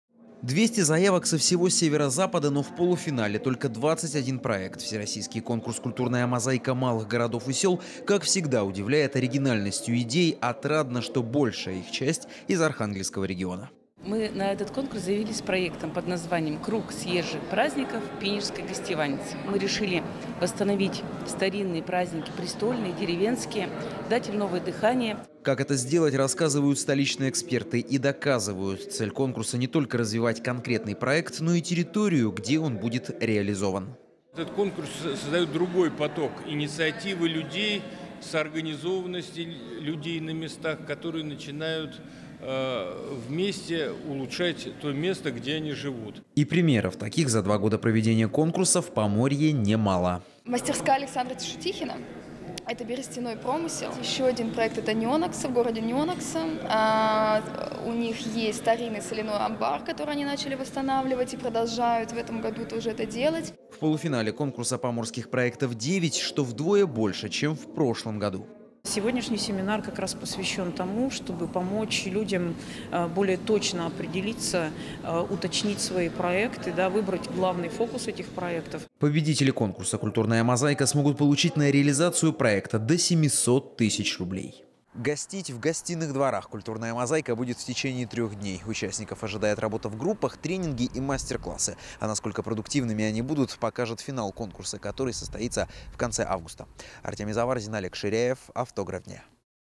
Семинар в Гостиных дворах 14-16.07.2015
Открытие образовательного семинара для полуфиналистов конкурса «Культурная мозаика малых городов и сёл» в 2015 году. Биржевой зал музейного комплекса Гостиные дворы (ул. Набережная Северной Двины, д. 86) Архангельского краеведческого музея.